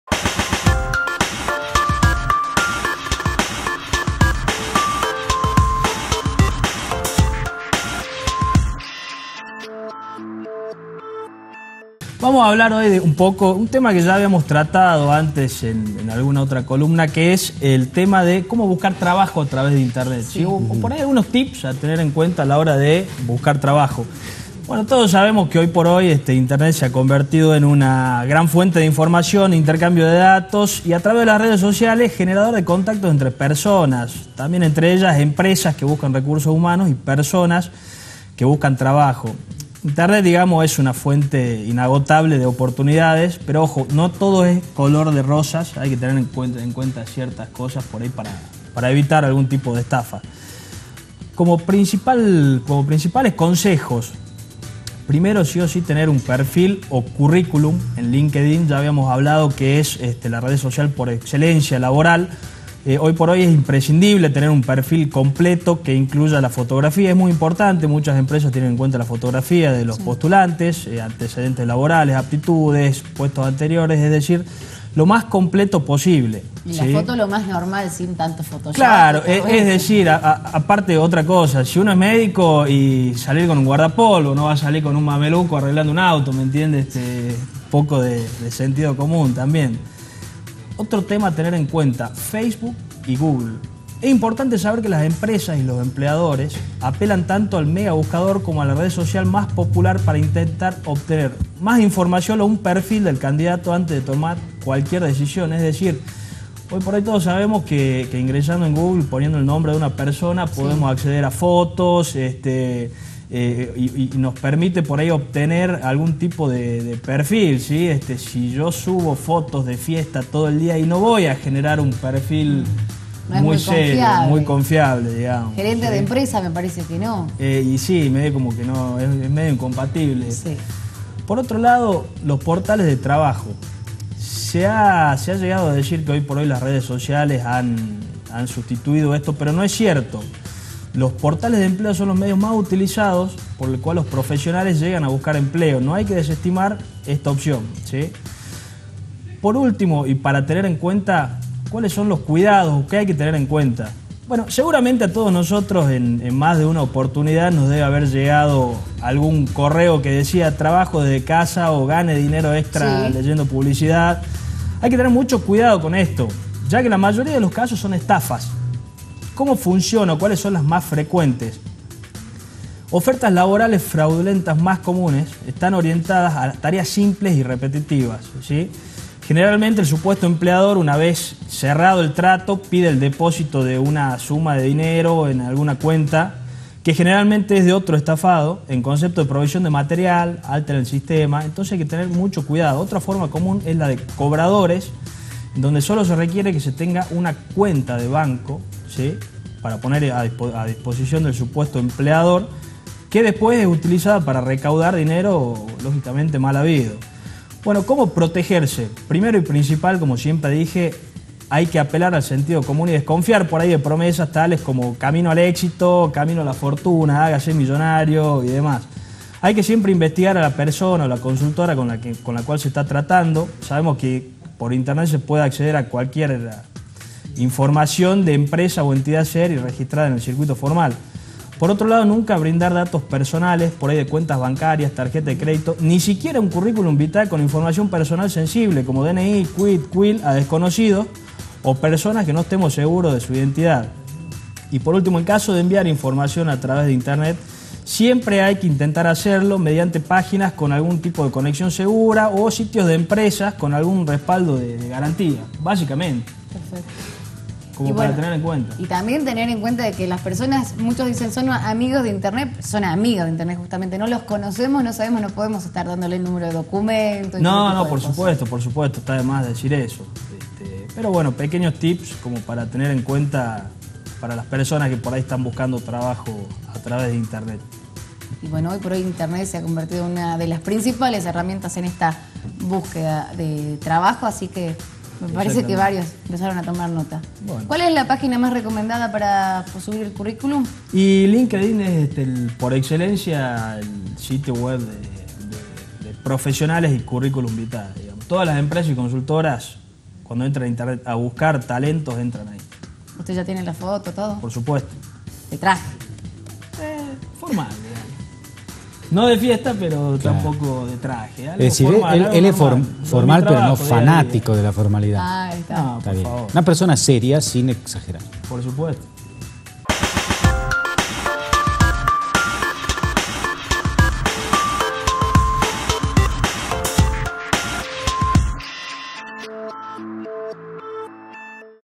¿Cómo buscar trabajo en internet?
La columna del especialista, Juan Pablo Possetti, brindando detalles sobre las potencialidades de la gran red, sin embargo, hay riesgos y Juan Pablo nos brinda consejos para evitar cometer errores.